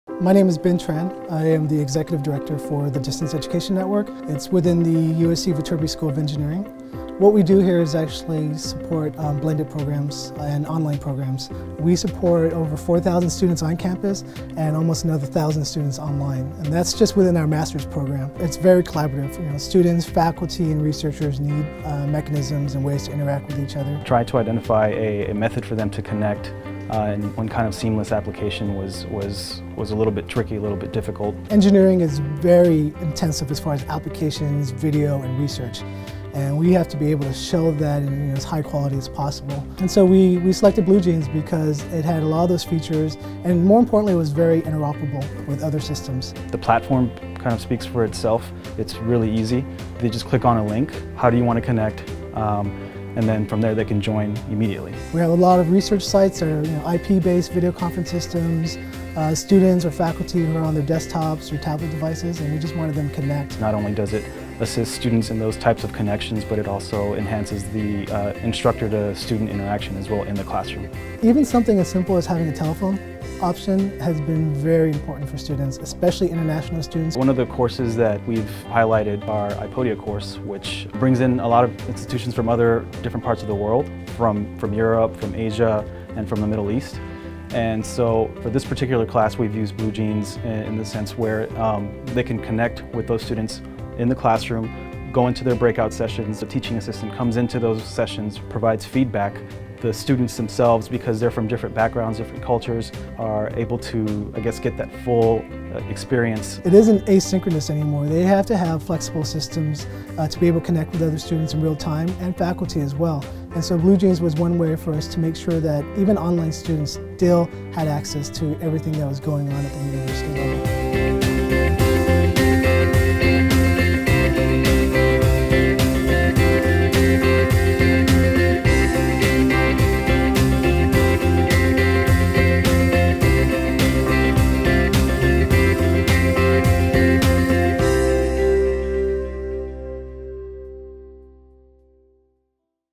BlueJeans Customer Story: USC Viterbi School of Engineering
BlueJeans allows online and international students to engage face-to-face with USC's on-campus classes.